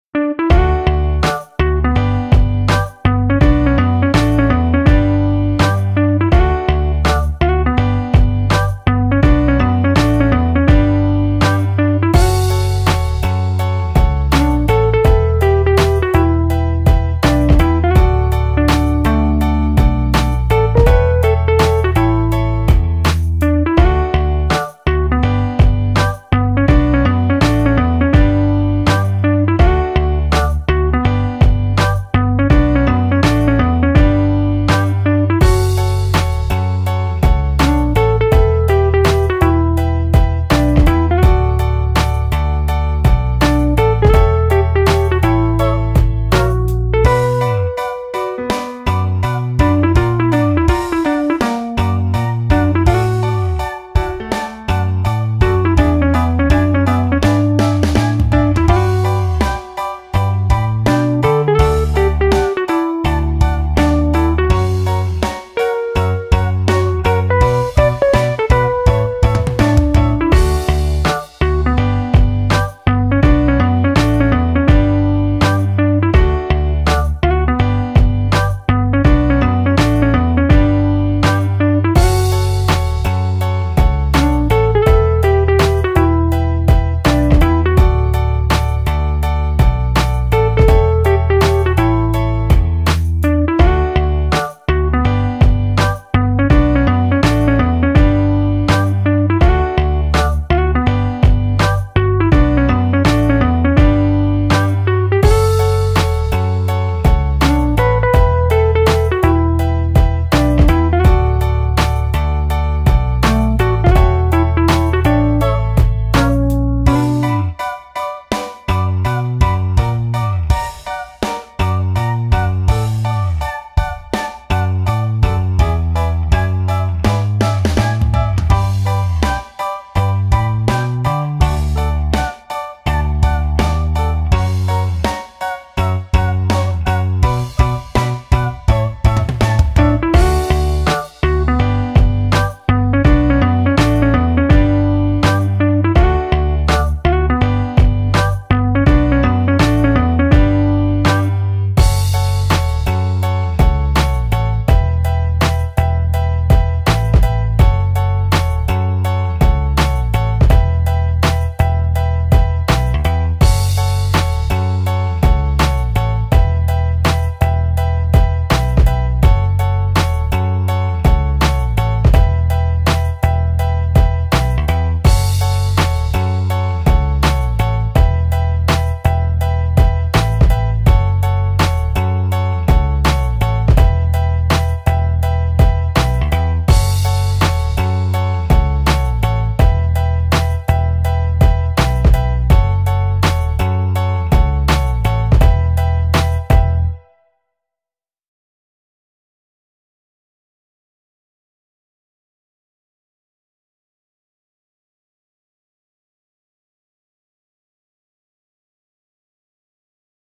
Time Lapse Drawing of RGBros: Water Bear
A time lapse of drawing the RGBros. comic: Water Bear